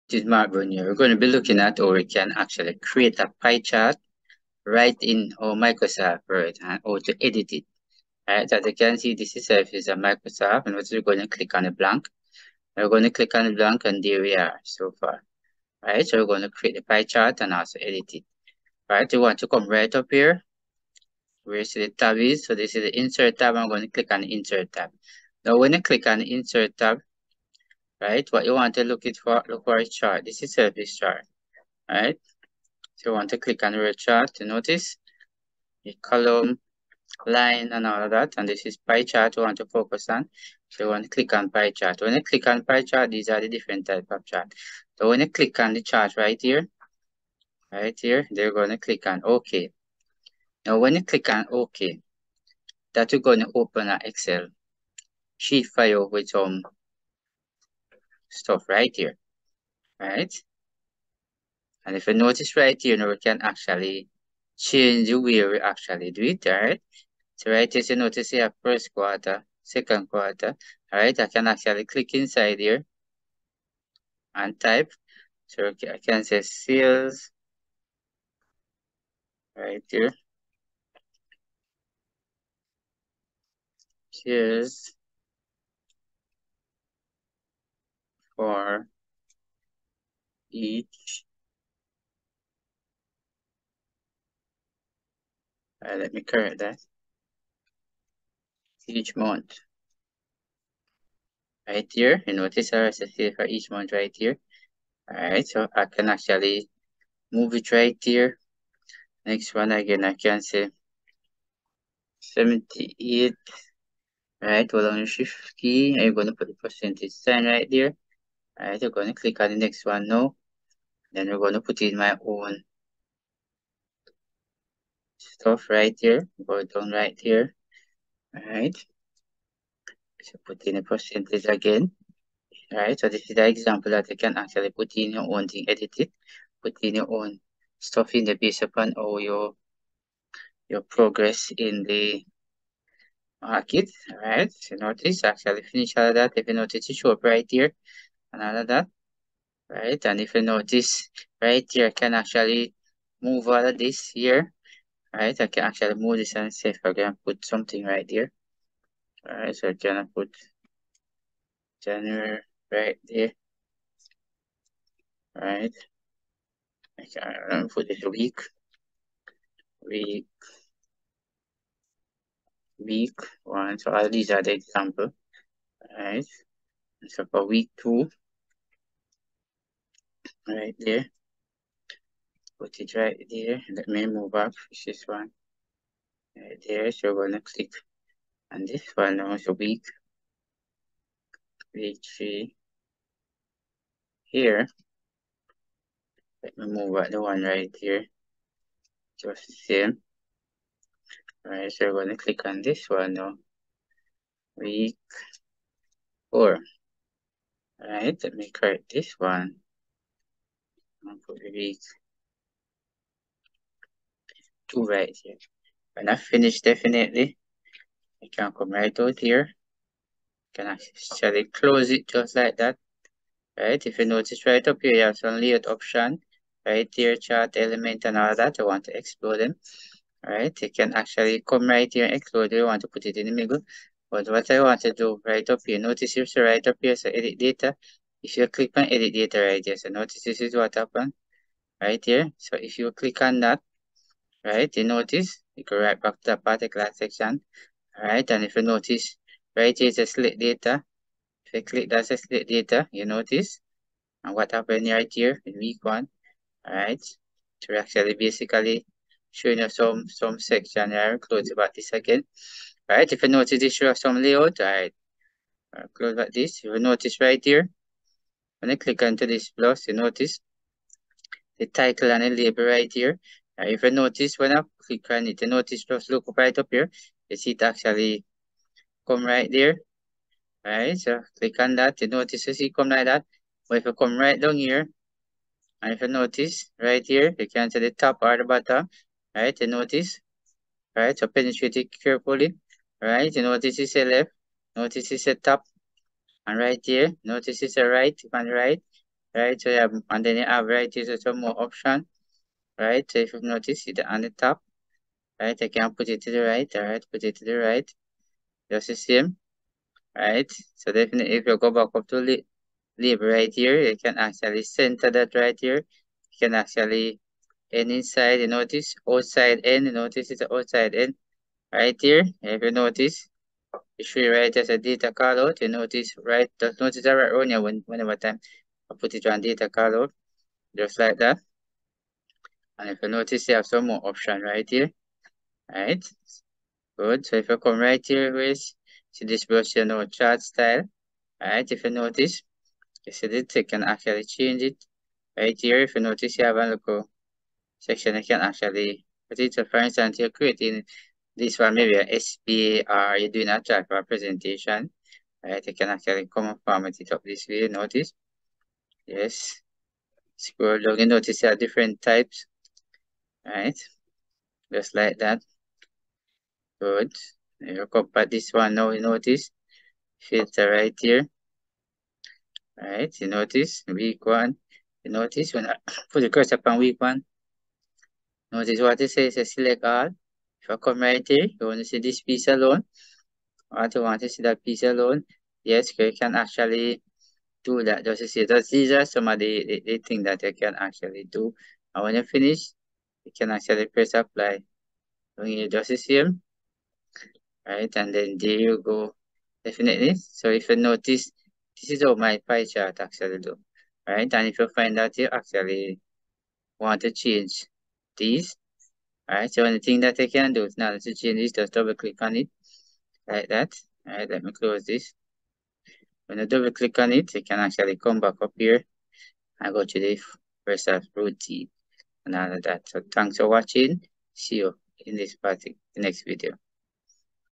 HOW TO CREATE PIE CHART AND EDIT IT IN WORD
In this step-by-step video, you will learn  how Pie charts communicate a snapshot of data. Word has all the tools you need to make a well-formatted pie chart.